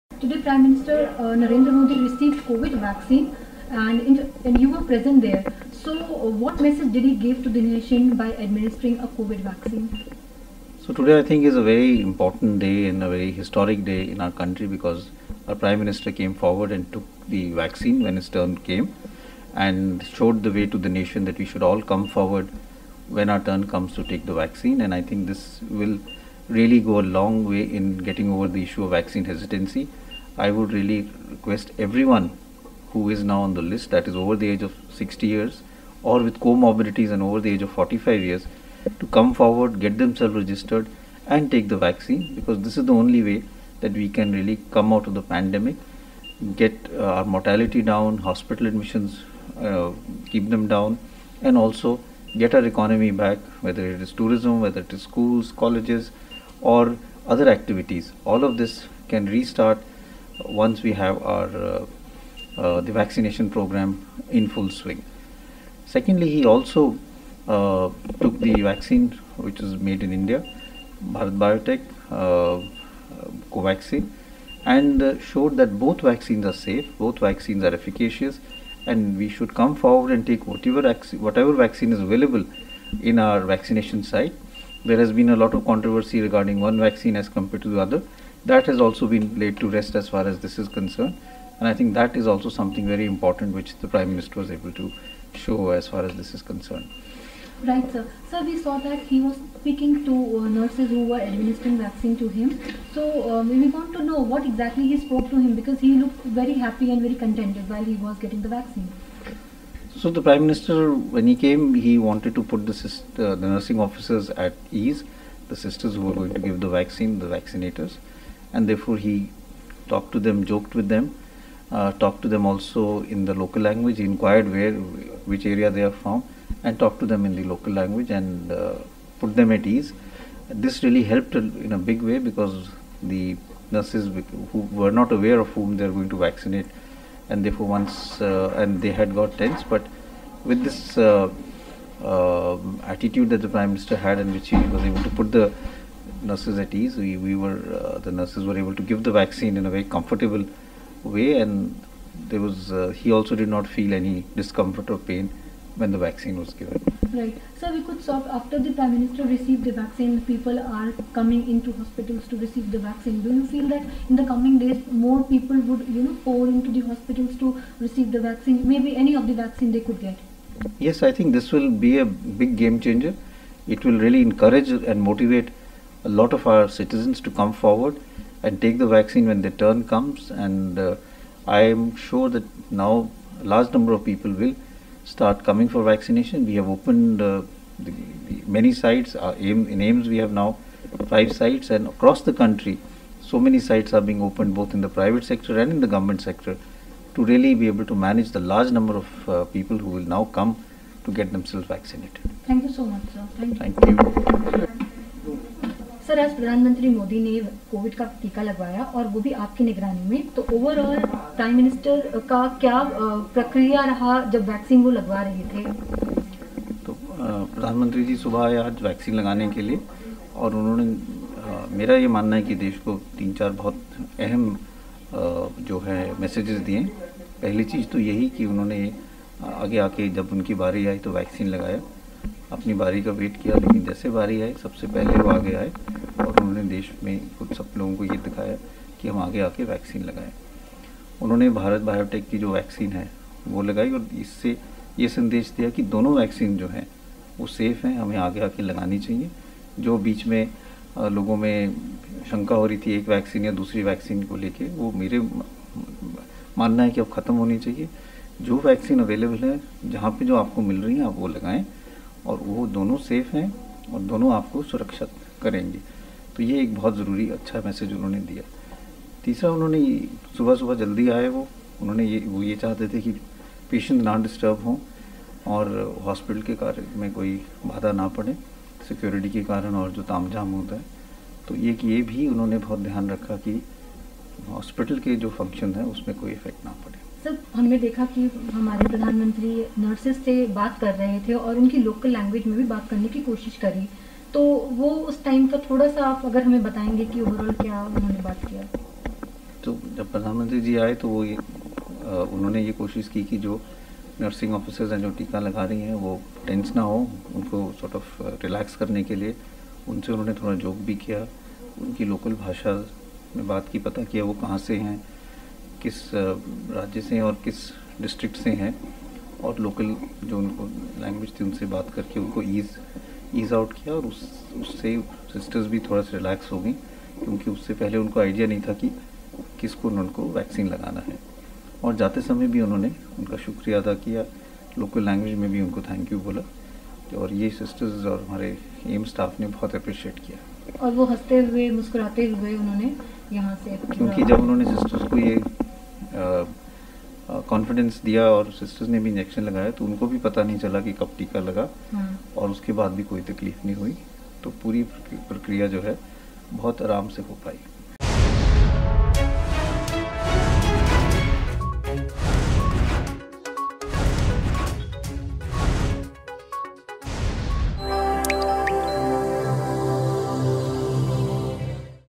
ସମସ୍ତଙ୍କୁ ଭ୍ୟାକ୍ସିନ ନେବା ପାଇଁ ପରାମର୍ଶ ଦେଲେ ଏମ୍ସ ଡାଇରେକ୍ଟର
ବର୍ତ୍ତମାନ ପରିସ୍ଥିତିରେ ସମସ୍ତେ ନିଜେ ସତର୍କିତ ରହିବା ଆବଶ୍ୟକ ଏବଂ ଟୀକା ନେବାକୁ ପରାମର୍ଶ ଦେଇଛନ୍ତି ଏମ୍ସ ଡାଇରେକ୍ଟର.
ଅଧିକ ଖବର ପାଇଁ ଡ଼ାଉନଲୋଡ କରନ୍ତୁ “Argus News” ଆପ୍